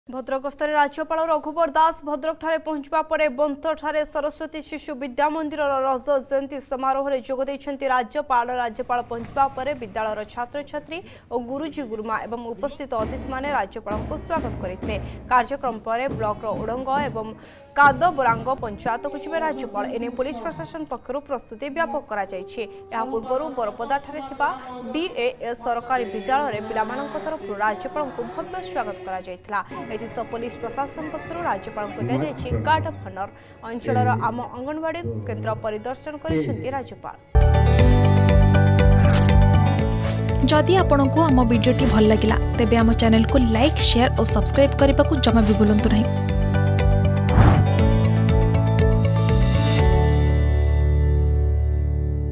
ଭଦ୍ରକ ଗସ୍ତରେ ରାଜ୍ୟପାଳ ରଘୁବର ଦାସ || Governor RaghubarDas visit Bhadrak || Argus News
ଭଦ୍ରକ ଠାରେ ପହଞ୍ଚିବା ପରେ ବନ୍ତଠାରେ ସରସ୍ଵତୀ ଶିଶୁ ବିଦ୍ୟା ମନ୍ଦିରରେ ରଜତ ଜୟନ୍ତୀ ସମାରୋହରେ ଯୋଗଦେଇଛନ୍ତି ରାଜ୍ୟପାଳ l